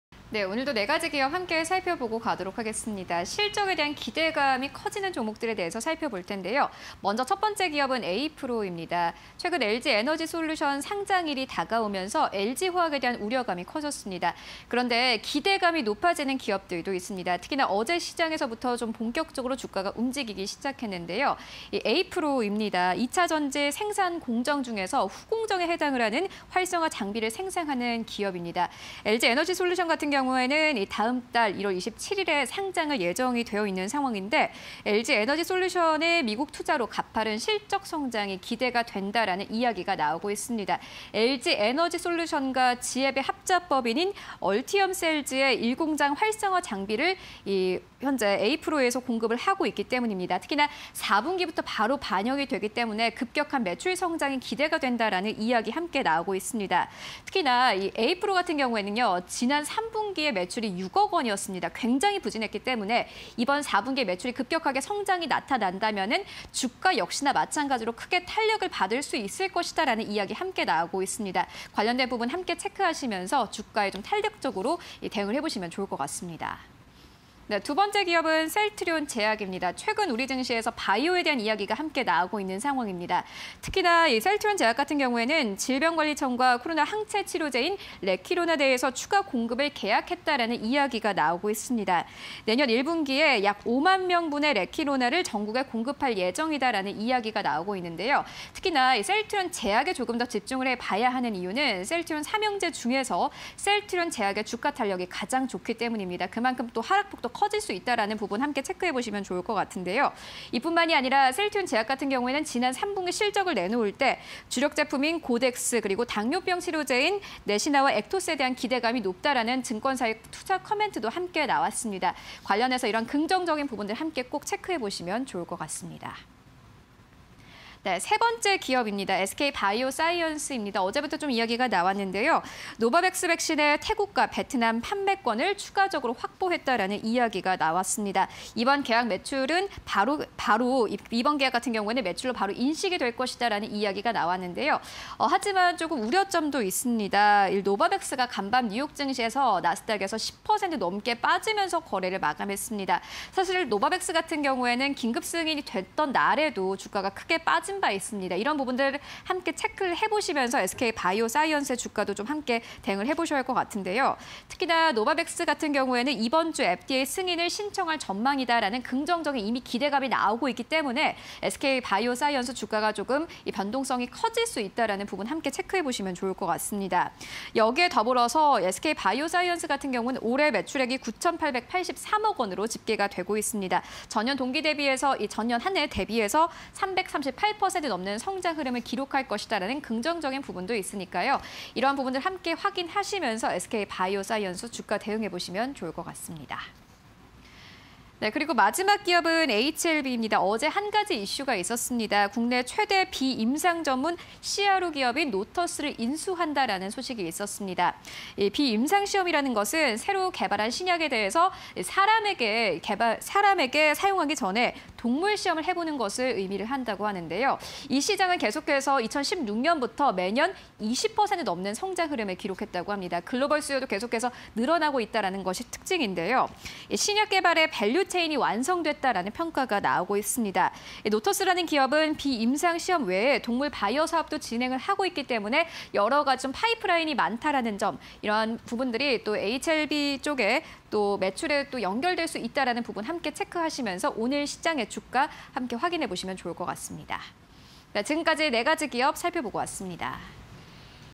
에이프로, 셀트리온제약, SK바이오사이언스, 에이치엘비_공시&뉴스 (20211228)
- 2차전지 생산공정 중 후공정 해당 활성화 장비 생산
- LG에너지솔루션, 美 투자로 가파른 실적 성장 기대
- 장비 공급 증가 전망이 핵심
- 얼티엄셀즈 1공장 활성화 장비 공급분 4분기부터 반영

- 질병관리청과 코로나 항체치료제 추가 공급 계약
- 주당 0.03주 주식배당 결정
- 주력 제품 고덱스·네시나·엑토스 기대감↑

- 노바백스 백신, 태국·베트남 판매권 추가적 확보
- 노바백스 백신, 이번 주 FDA 승인 신청 전망
- 올해 매출액 9,883억 원… 전년대비 338%↑

- 국내 최대 비임상 전문 CRO 노터스 인수
- 신약개발 밸류체인 완성됐다 평가 나와
- 비임상시험, 새로 개발 신약 후보물질 동물 시험 의미
- 노터스, 비임상시험 외 동물 바이오사업도 진행